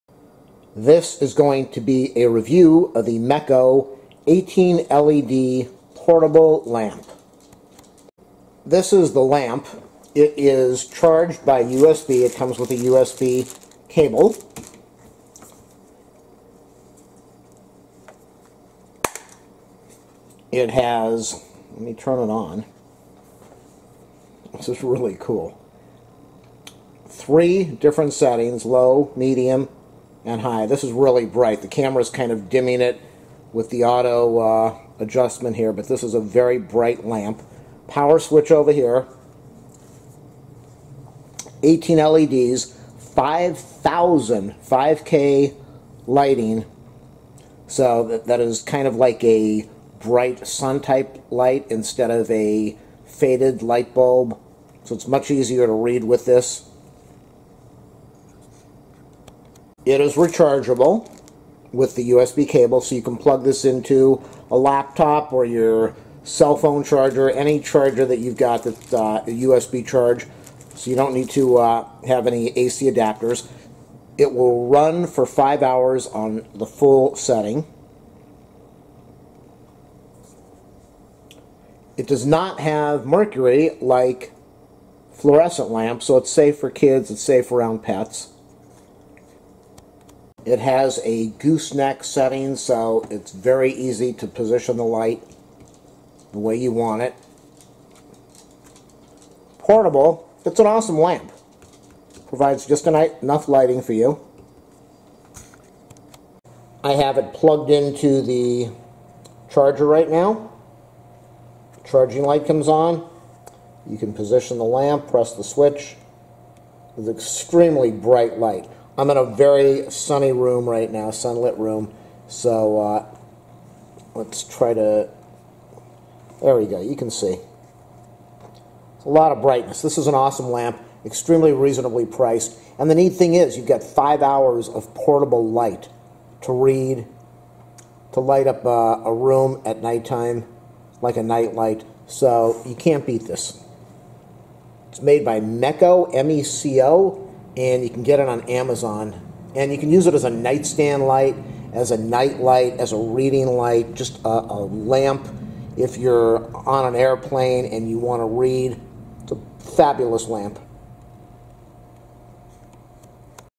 LED LAMP Meco BATTERY powered Portable LED LAMP 5 HOURS OF LIGHT! review
Portable LED Desk Lamp Travel Light 3-Level Dimmable Brightness Nightstand Lamp Touch Sensitive Control USB Rechargeable 1000mah Li-on Battery Flexible Lamp For Besides Reading. This is where I got it. I love the red but also comes in Orange